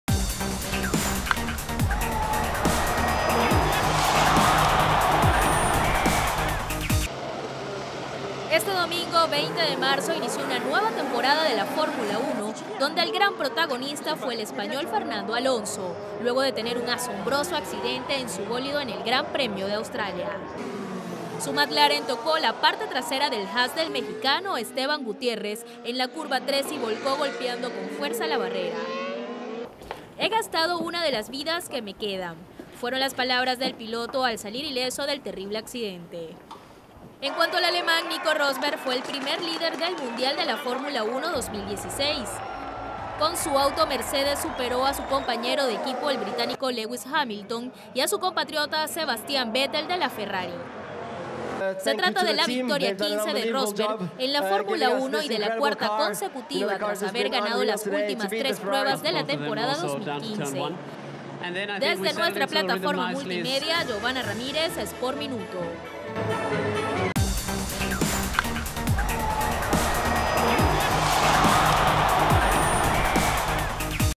El brutal accidente de Fernando Alonso en el GP de Australia
El vehículo del doble campeón mundial asturiano salió despedido por los aires, dando dos vueltas en un aparatoso accidente